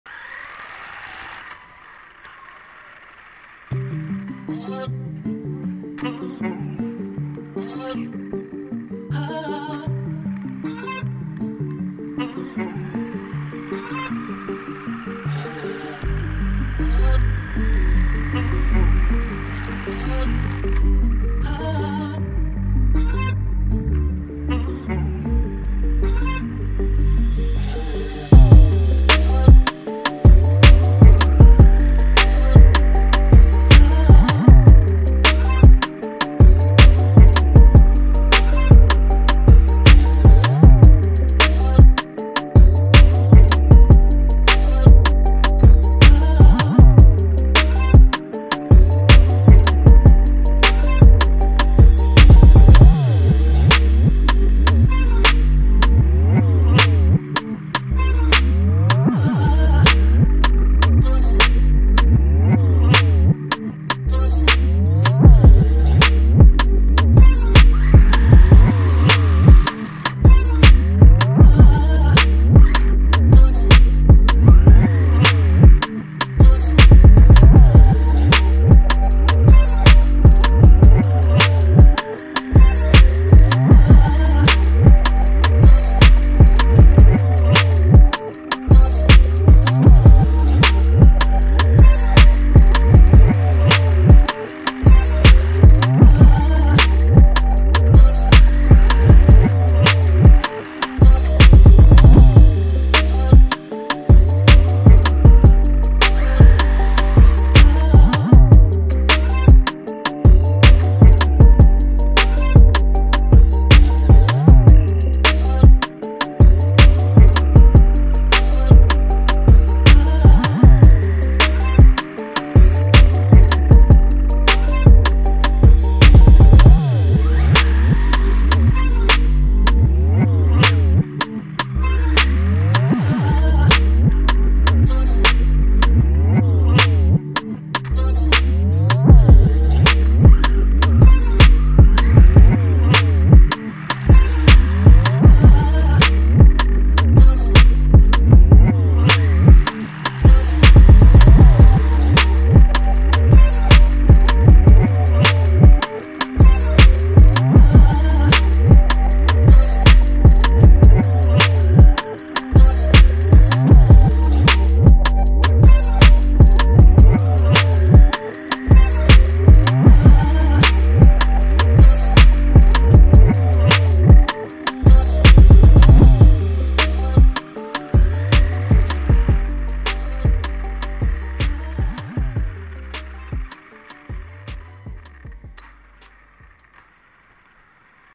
RC FPV Camera Car First Test
RC Camera Car explores Tonsley Technology Park